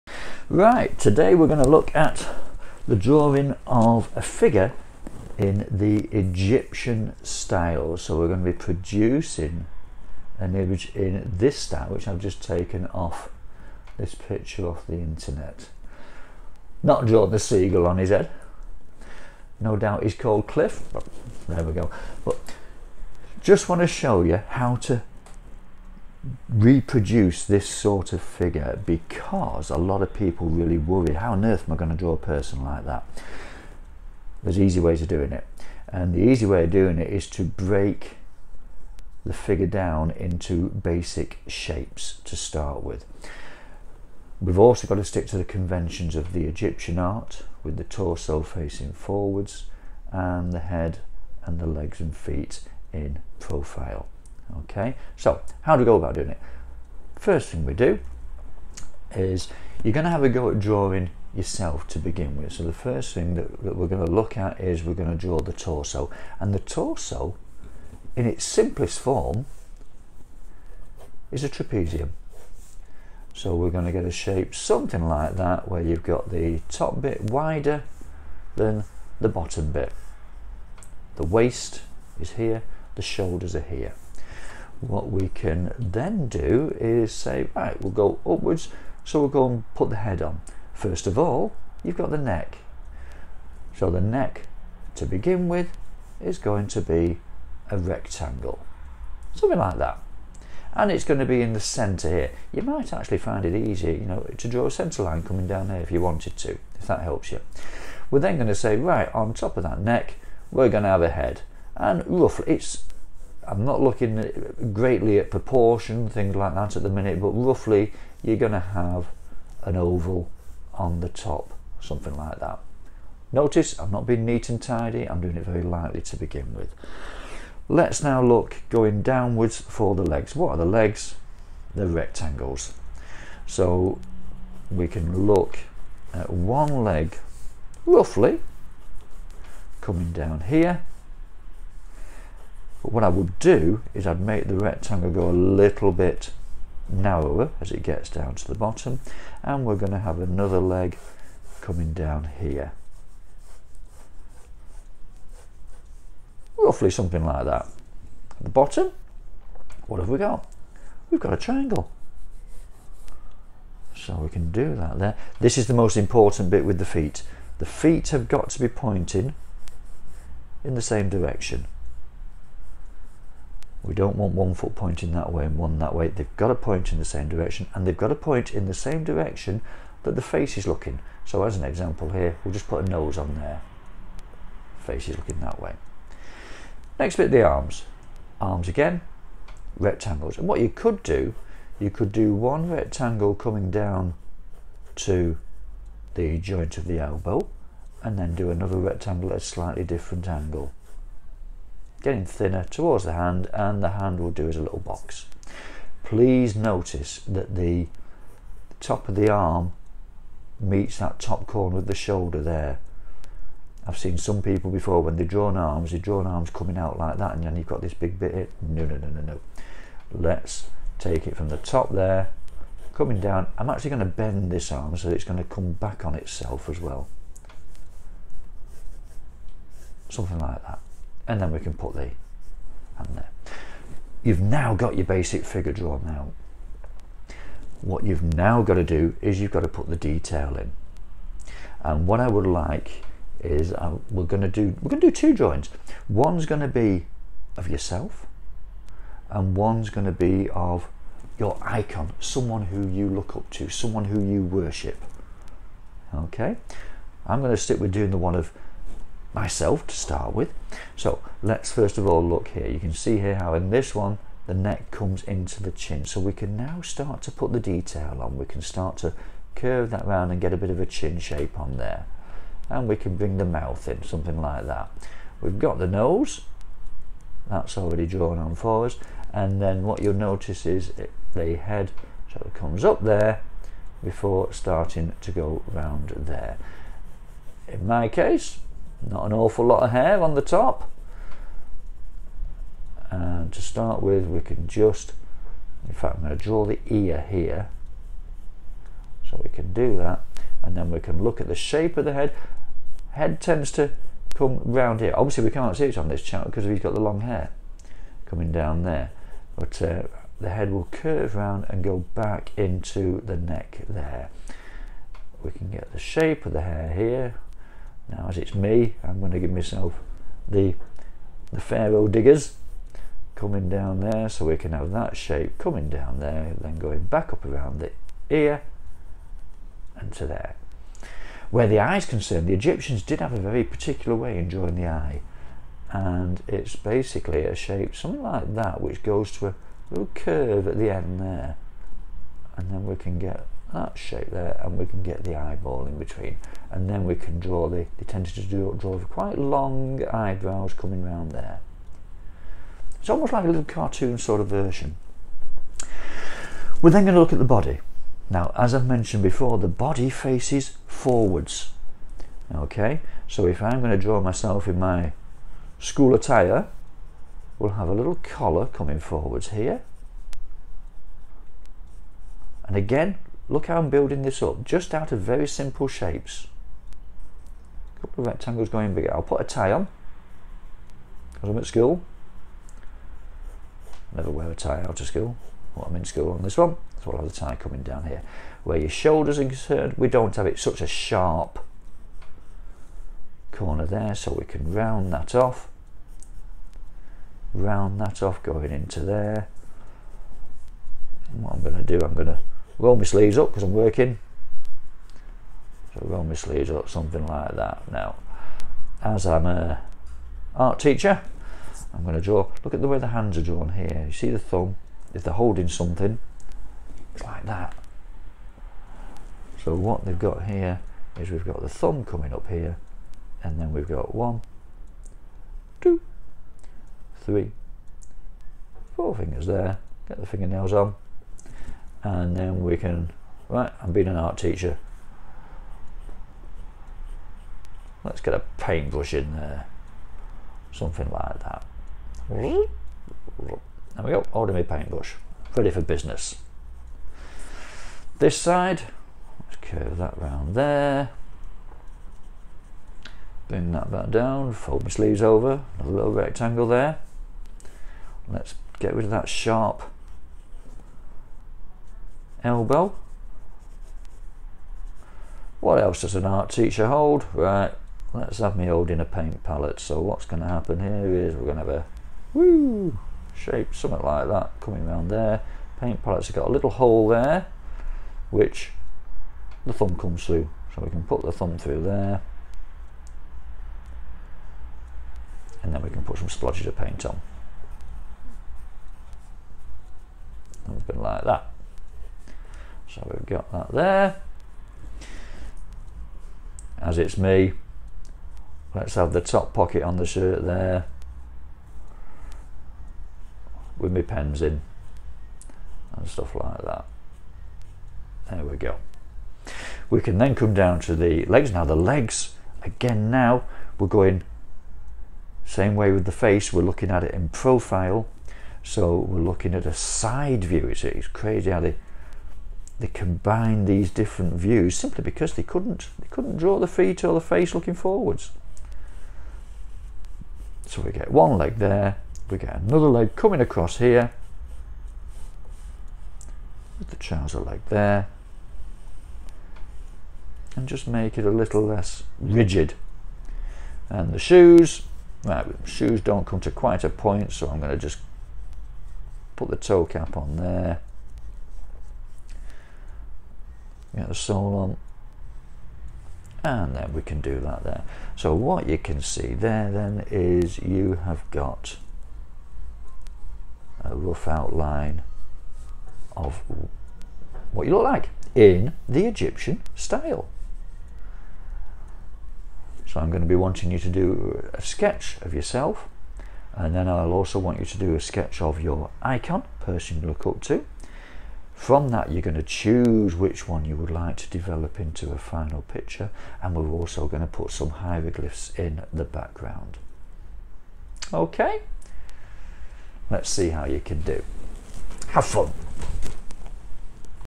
Egyptian Art 2 - The Figure
How I draw a figure in the Egyptian style.